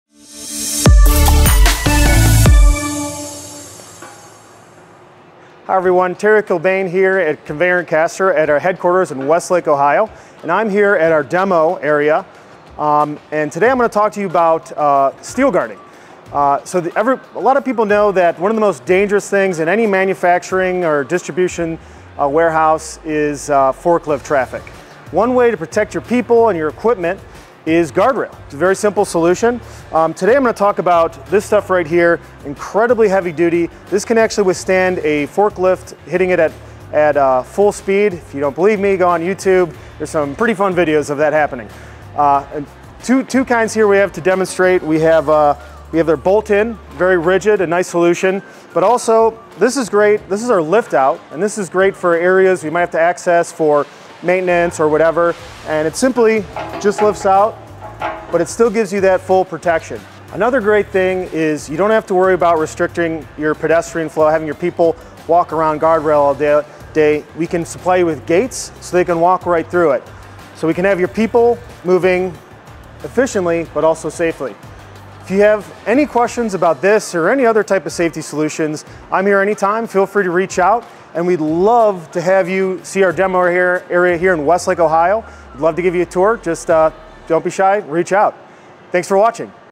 Terry's Takes - Guard Rail | Conveyer & Caster
In this edition of Terry's Takes, we are talking about protecting foot traffic in forklift areas with Guard rails!

Like this solution? Reach out today!